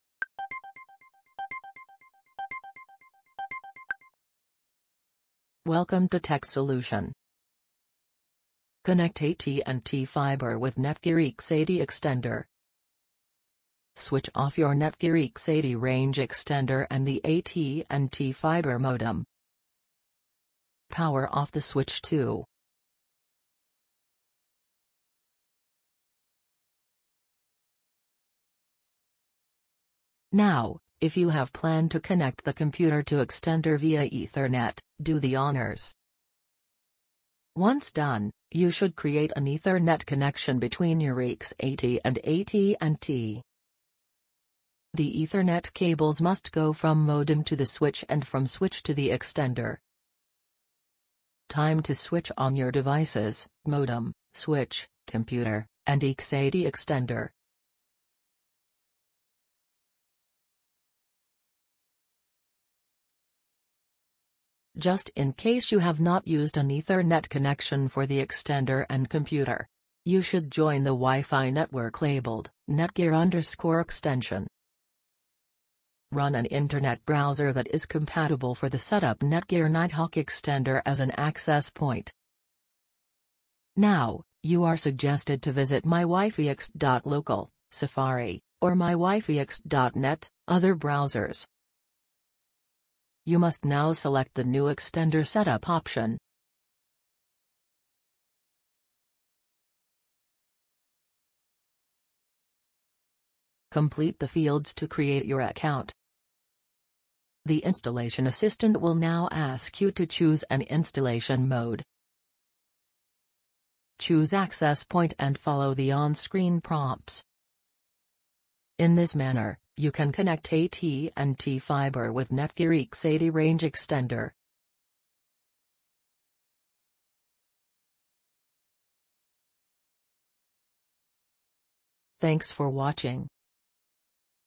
Connect AT&T Fiber with Netgear EAX80 extender
Learn how to easily extend your AT&T Fiber network with the Netgear EAX80 extender! In this tutorial, we'll guide you through the step-by-step process of connecting your AT&T Fiber router to the Netgear EAX80 extender for seamless Wi-Fi coverage throughout your home. Say goodbye to dead zones and enjoy high-speed internet in every corner.